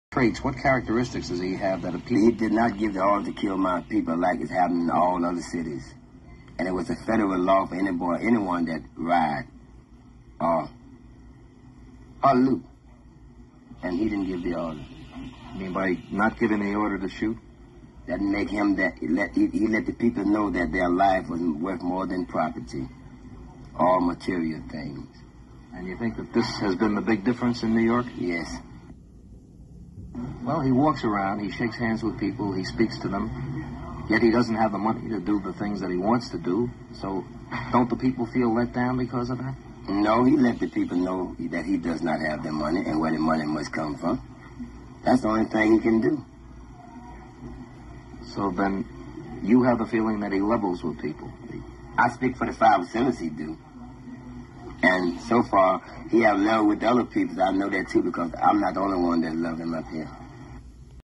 A brief interview with Bro Clarence 13X aka Father Allah
The NBC Press (1968) interviews The Leader of The Five Percent Nation of Islam aka Five Percenters about matters concerning John F. Kennedy and Dr Martin L. King assassinations.